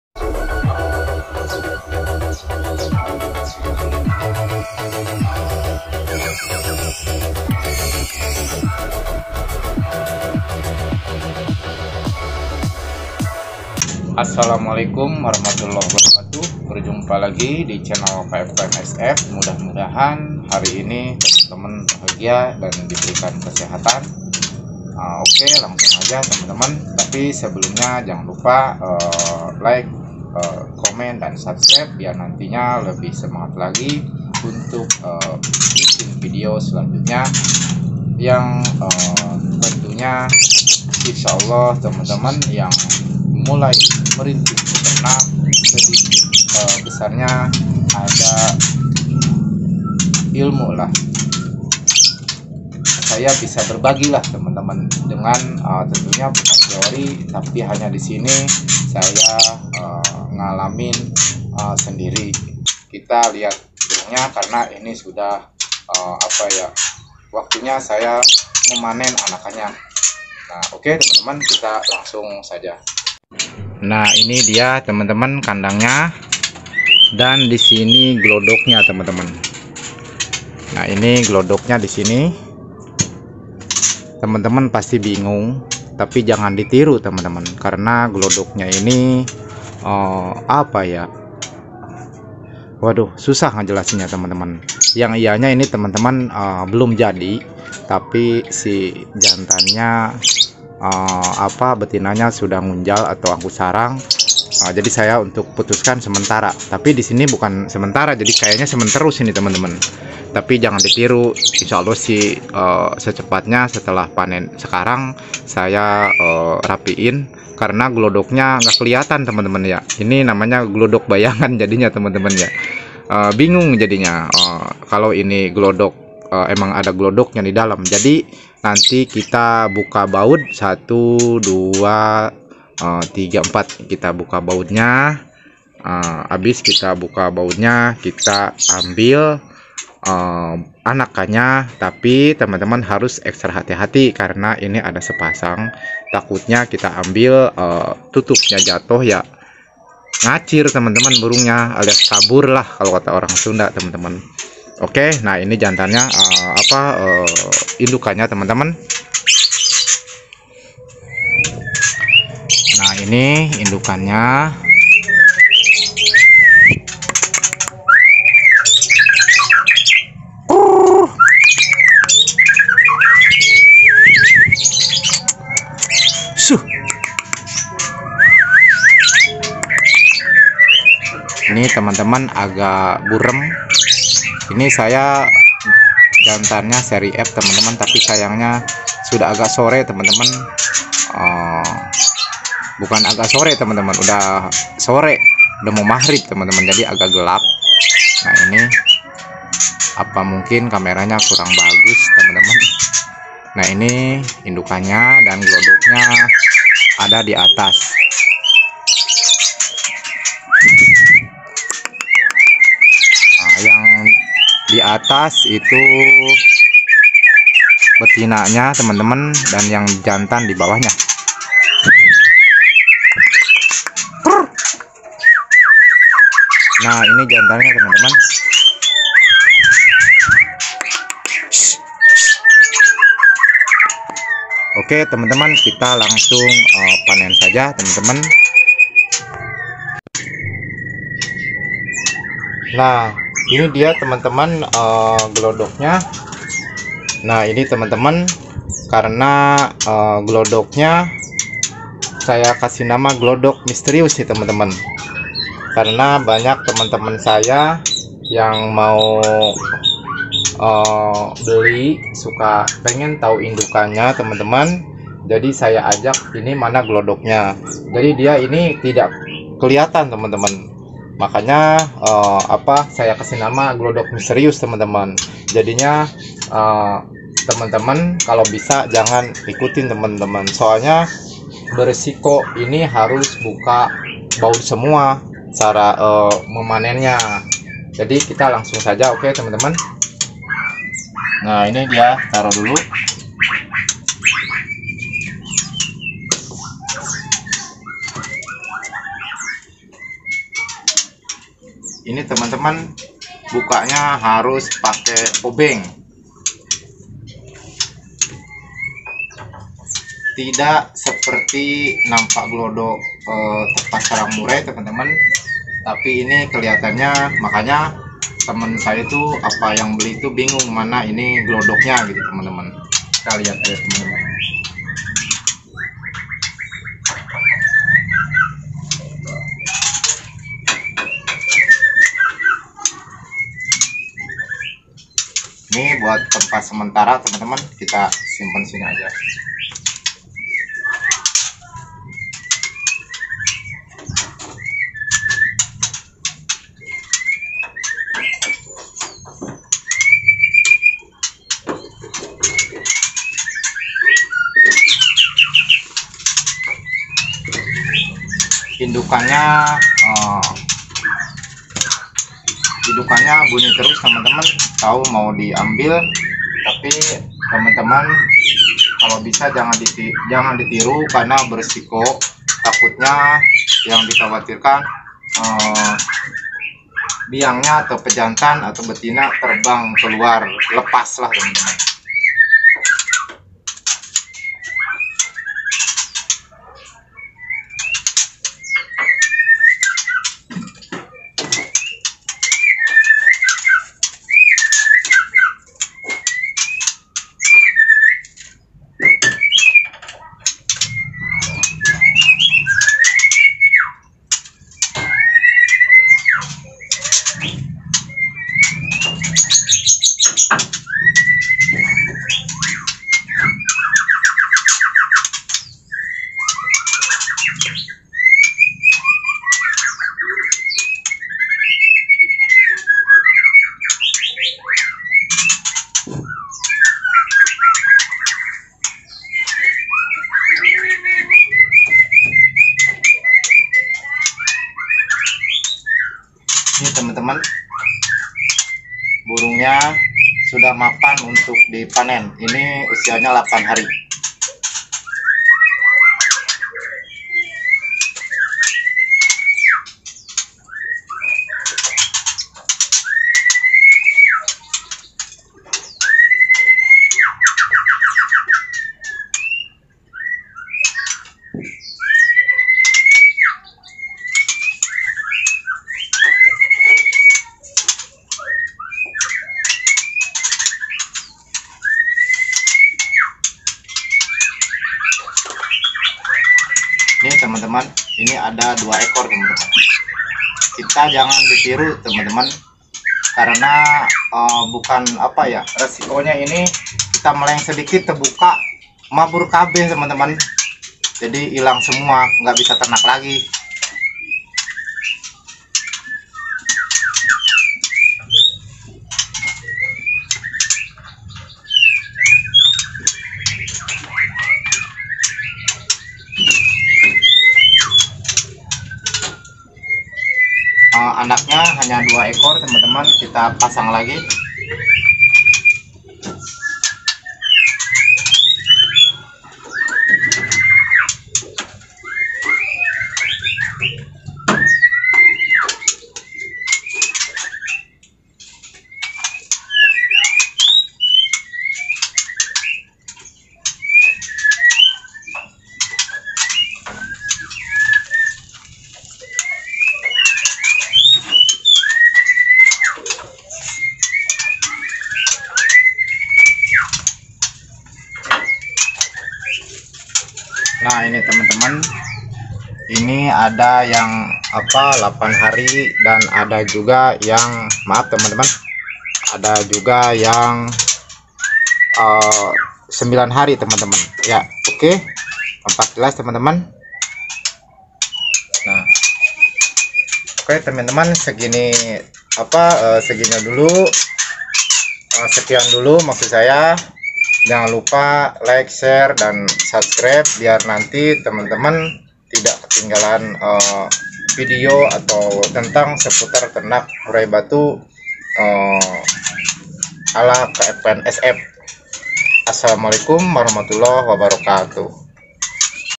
waktunya panen MURAI BATU Glodok MISTERIUS,,
panenmuraibatu GLODOK MISTERIUS ,, anda pasti penasaran krena ini hnya satu"nya,,dengan sistim kawin cabut jeulas lebih menguntungkan,,pakan harian dan jantan tidak harus banyak